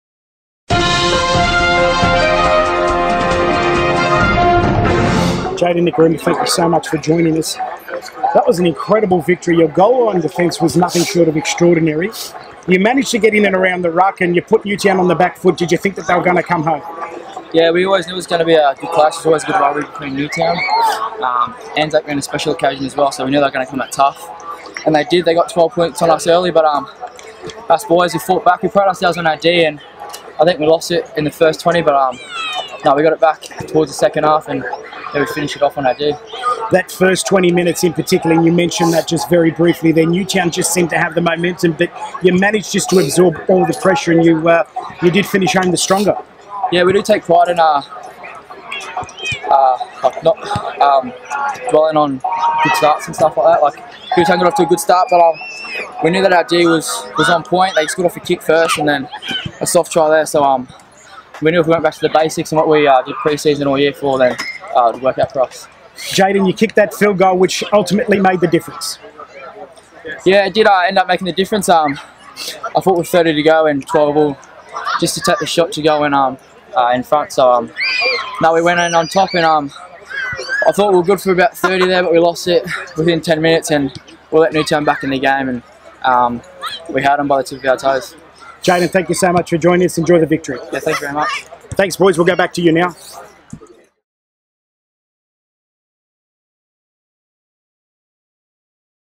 Steele Sports - NSWRL ISP Round 8 - Post Match Interview - Jayden Nikorima - Wyong Roos
Alby and the Steele Sports team catch up with Jayden Nikorima and chat about Wyong's win over the Jets and his game changing field goal.